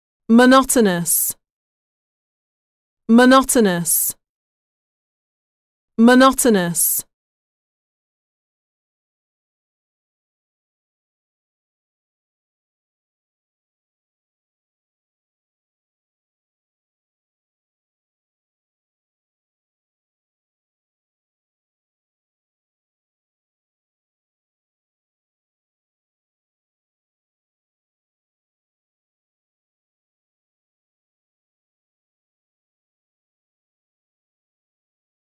영어 단어 어원으로 쉽게 공부하기- monotonous etymology [voca 1004]
짧은 유튜브 동영상을 통해 "monotonous" "monotone" "monotony" 어휘를 배워보세요.